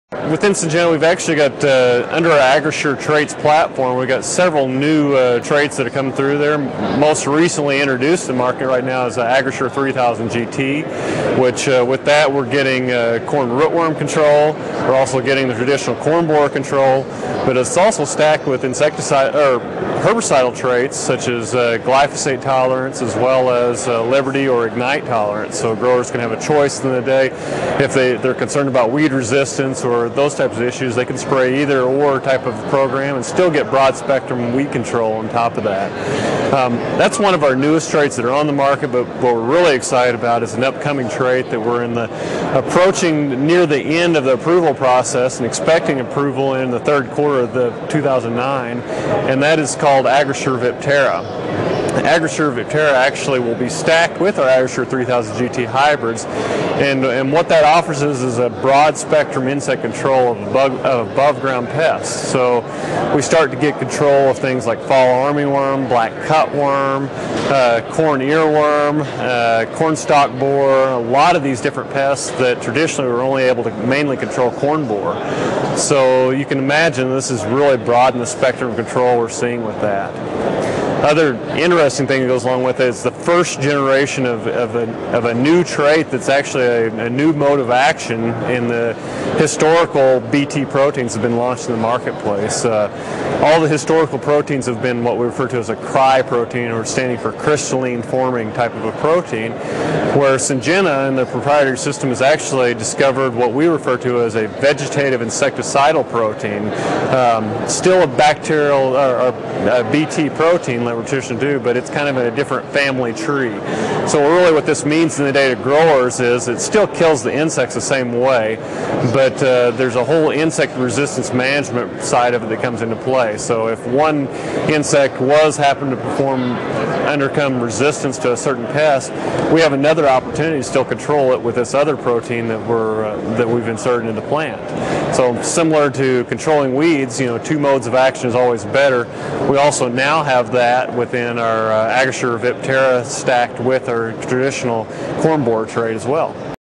New AgriSure Products from Syngenta Seeds
Bruce Battles from Syngenta Seeds talks about new traits in corn hybrids.